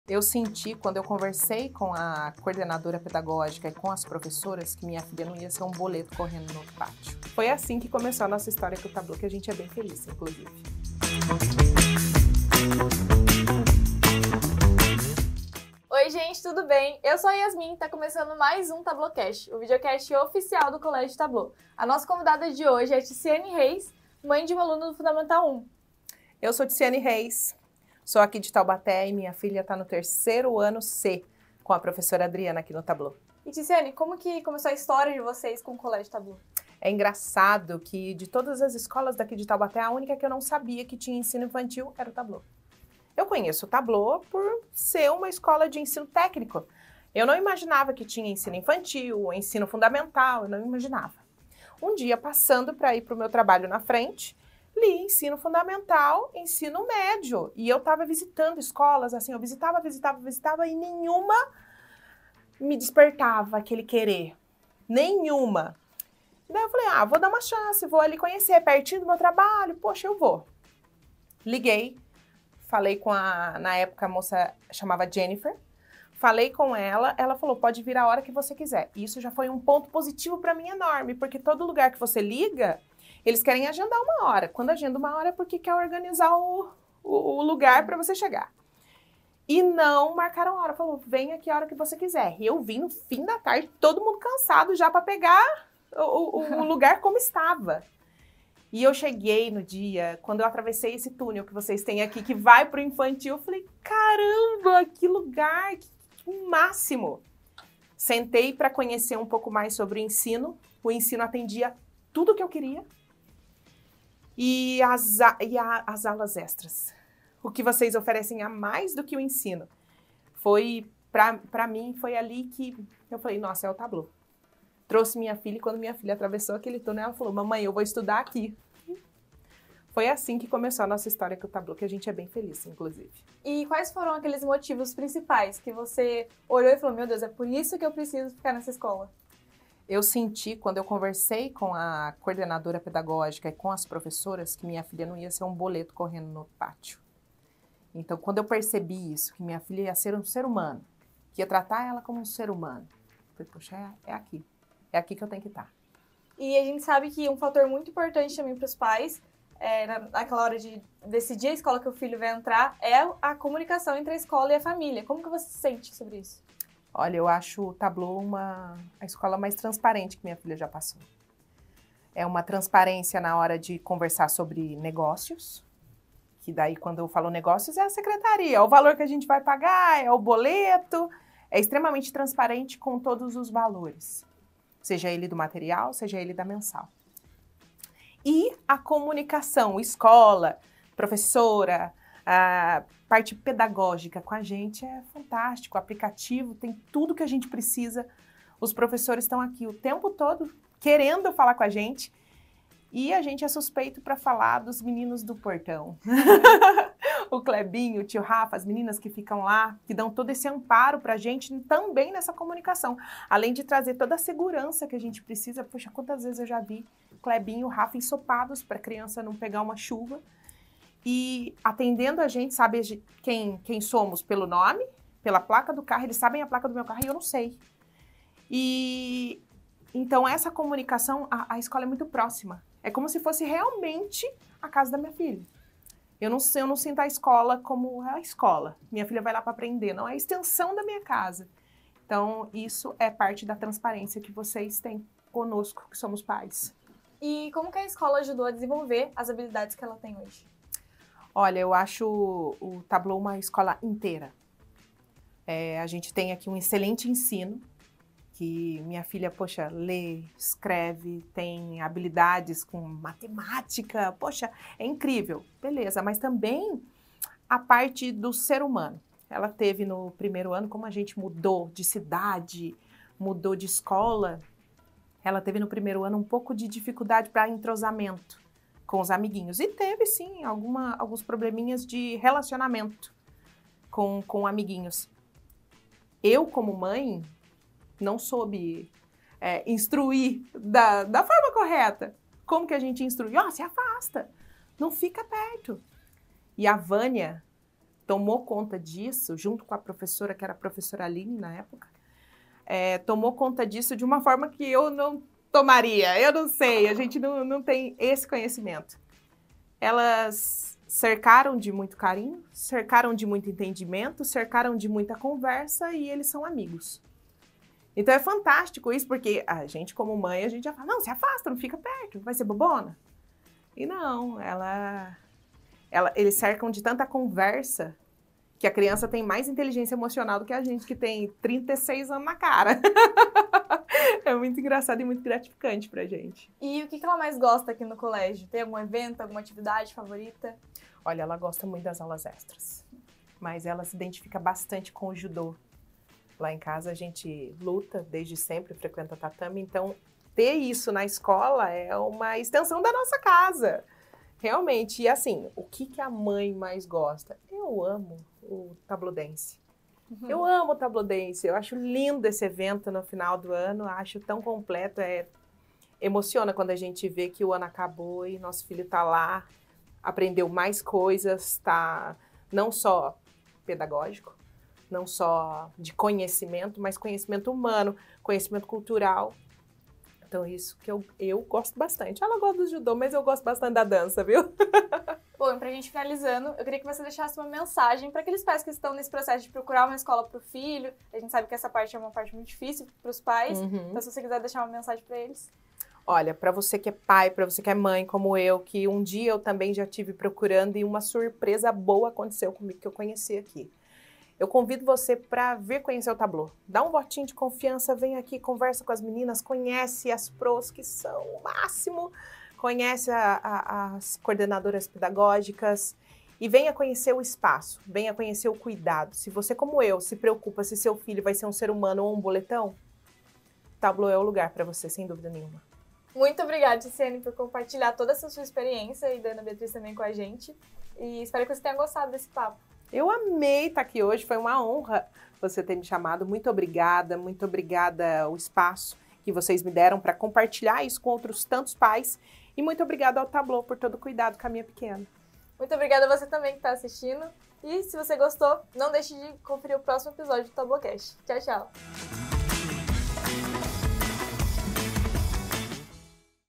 TICIANE REIS - TableauCast #8
No TableauCast de hoje, recebemos uma convidada especial: Ticiane Reis, mãe de um aluno do Fundamental I 🎙️

Ela compartilha sua trajetória com o Tableau, a comunicação próxima com nossa escola e como a educação aqui contribuiu para o desenvolvimento das habilidades de sua filha ✨

Uma conversa inspiradora sobre parceria e crescimento! Vem dar o play! ▶️